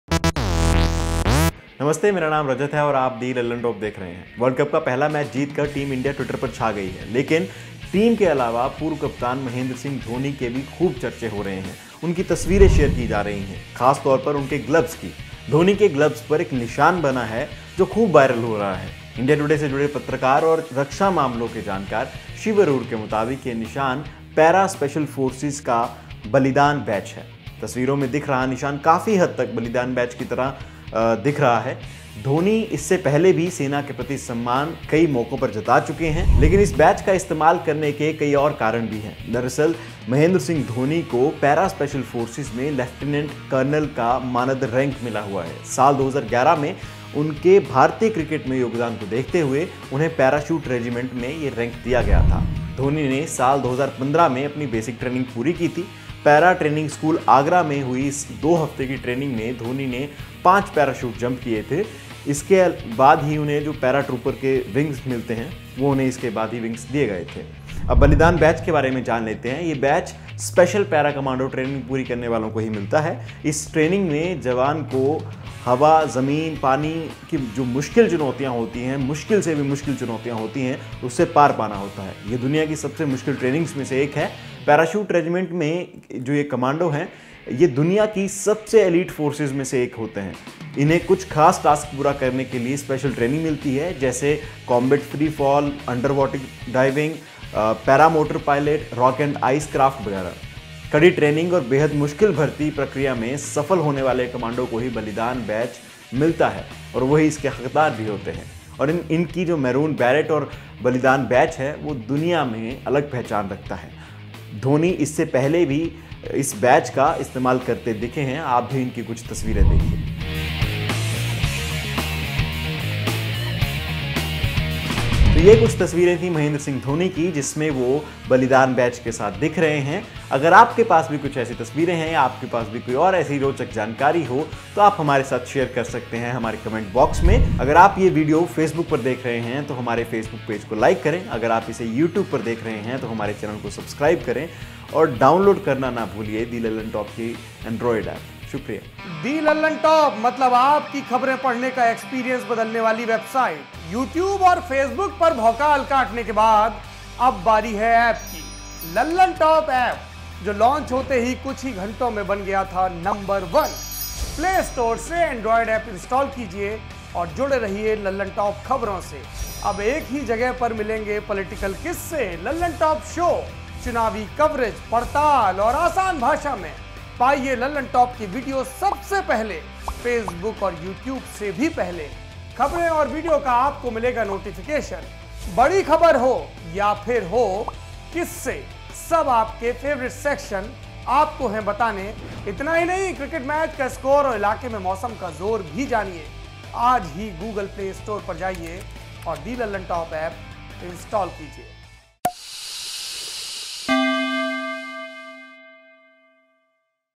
S Africa के खिलाफ़ Match में MS Dhoni के Gloves पर दिखा 'बलिदान' बैज आर्मी से जुड़ा है
MS Dhoni's viral glove pic has Indian Army's Balidaan insignia on it.

Edited By: Roohani